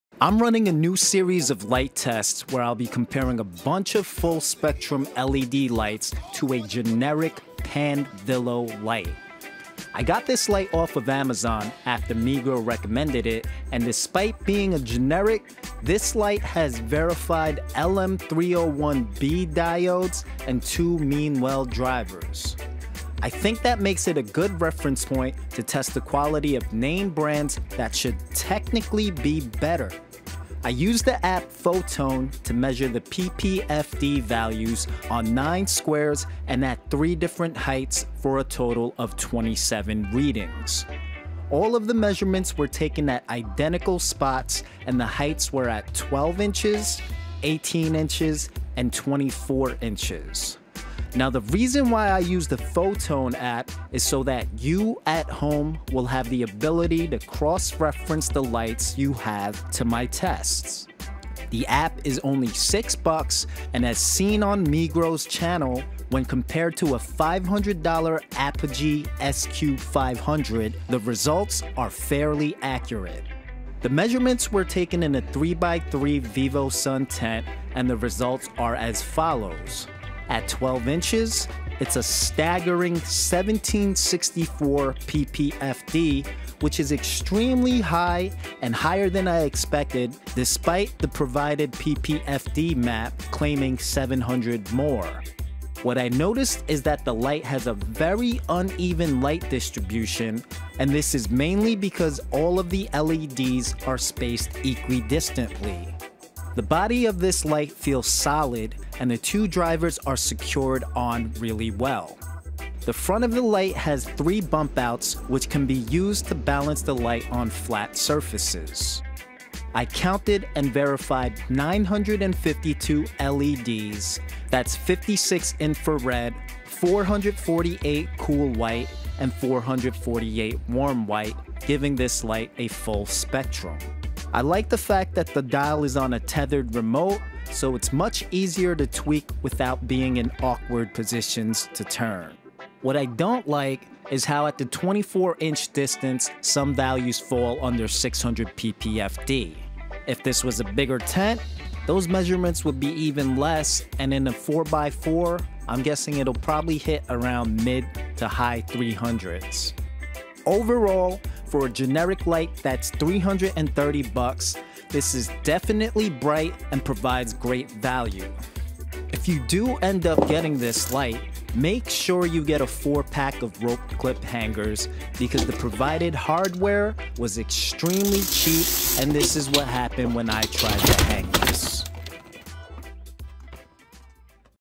PANDVILLOW LED Grow Light 400W - Generic Test and Review - LM301B & Meanwell Drivers
As seen in my best LED grow lights videos, I claim that all lights are pretty much the same as long as the drivers are good and they have either LM301B or LM301H diodes. This generic Pandvillow light does not disappoint and the light readings are super high and way more than necessary. The issue is the poor light distribution and since the LED's are spaced out evenly, the light is extremely intense in the center and really drops down significantly as you get further out. It's still and extremely powerful light for the price.